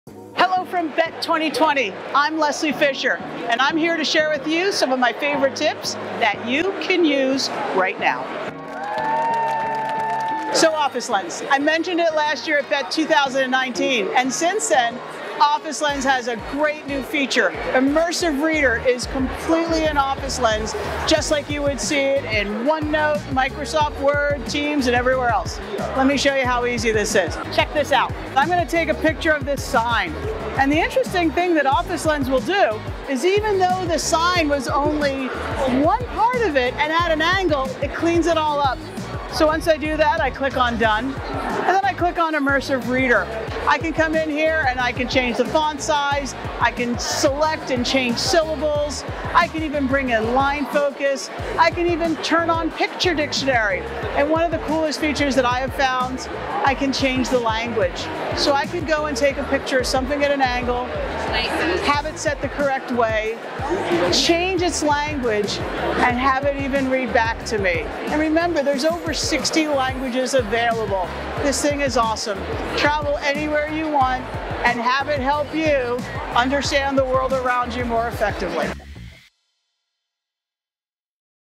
How to document the world around you with Office Lens!
Office Lens for iOS and Android both now have the full Immersive Reader experience integrated with the latest updates for both iPhone and iPad. Office Lens on Android (all platforms) will be shipping a similar update in spring. Office Lens is a free mobile scanning app. It offers a great way to capture text from a document or elsewhere without manually having to retype it.

👇 Additional Office Lens Resources 👇

🎓 Check out this free course to learn about Office Lens and how it can be used in classrooms and schools